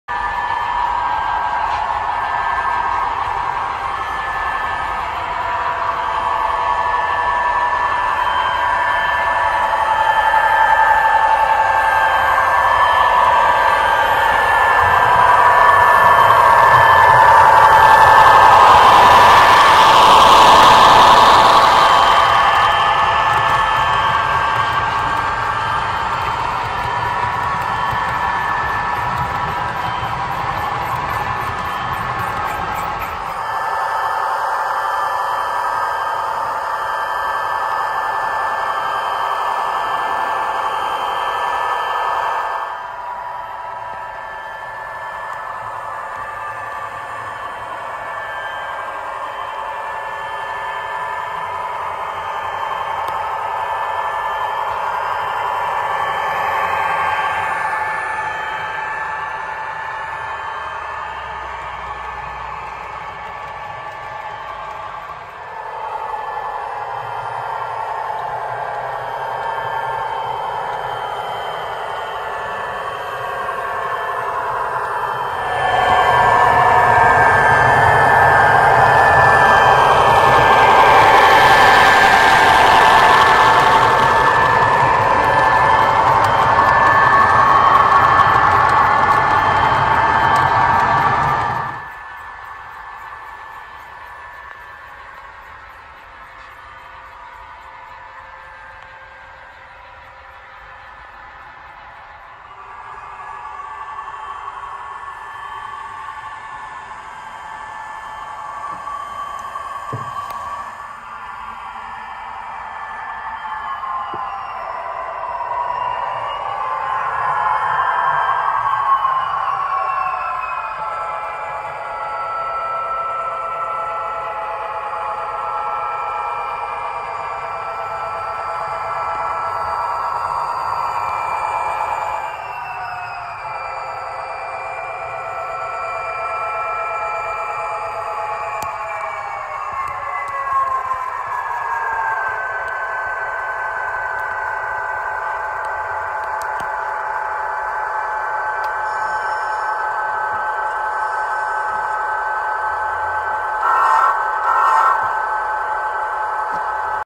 Helix, Staging Deck & Return Loop - Complete Run...
Here is a short video of the inaugural run through the helix, staging yard and return loops on the layout.  The staging deck and top deck are now joined and this will ad a lot more operational value to our operating sessions.